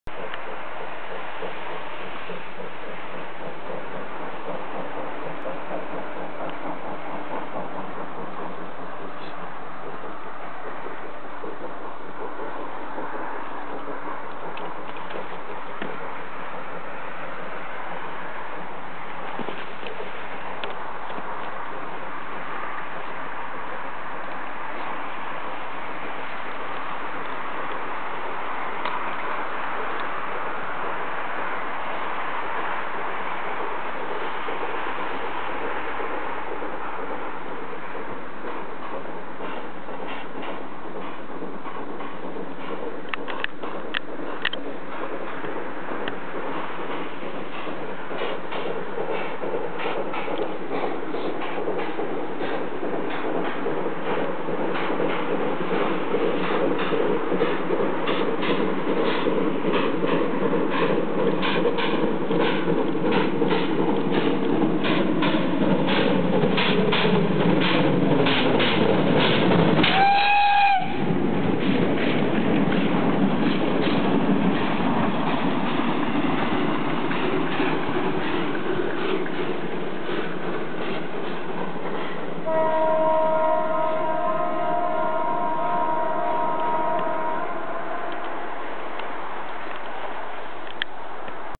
Pt47-65 Trzebaw (Rp1)
Skład R-77324 relacji Poznań Główny-Wolsztyn rozpędza się po postoju na stacji Trzebaw Rosnówko. Skład nagrany z nieczynnego mostu w tejże miejscowości. Pozdrowienia dla załogi parowozu, współtowarzyszy, wszystkich MK oraz nieznanego MK który pojawił się na krótko przed przejazdem składu